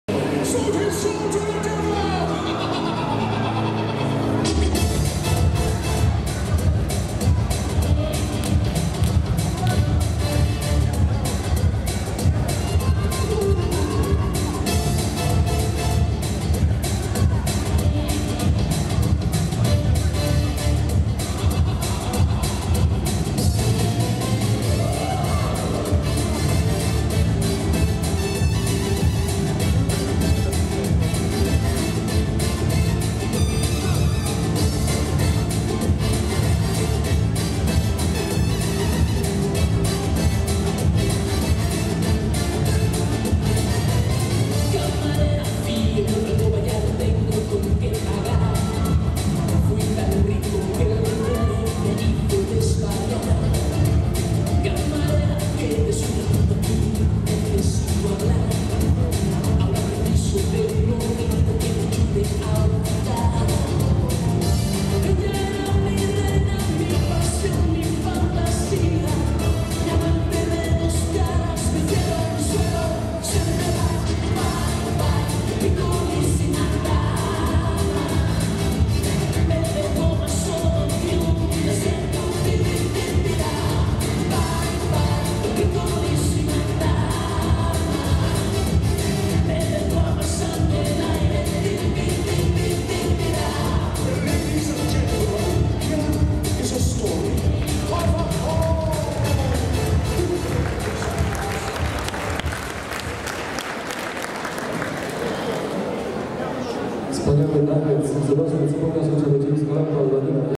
MELNIK Vladislav - SHURILOVA Daria, Final, Rock 'n' Roll-Juniors
Russian Federation, 3. place